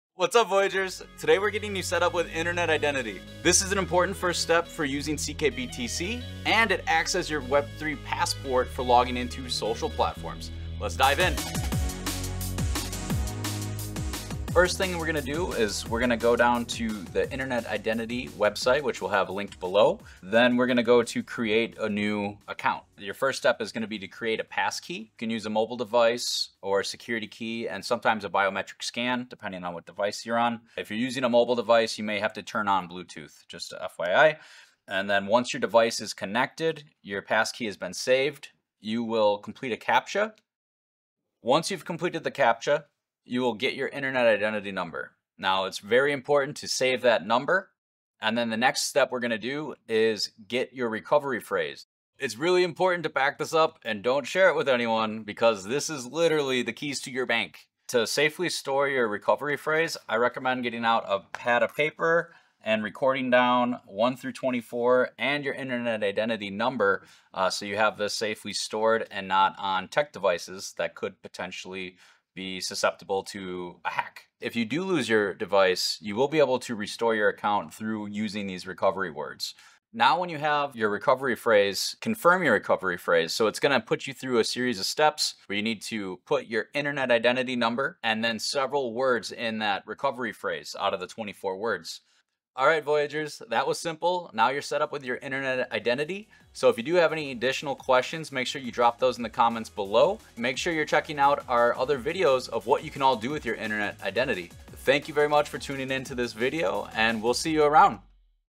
How To Set Up Your Internet Identity
In this video, Kyle Stoflet shows you how to set up your Internet Identity!

For additional help, check out the Identity support page 🔗

A Different Perspective on Crypto | Chris Giancarlo | The Blockchain Sessions

Ending Bitcoin Tribalism? | Kyle Langham | The Blockchain Sessions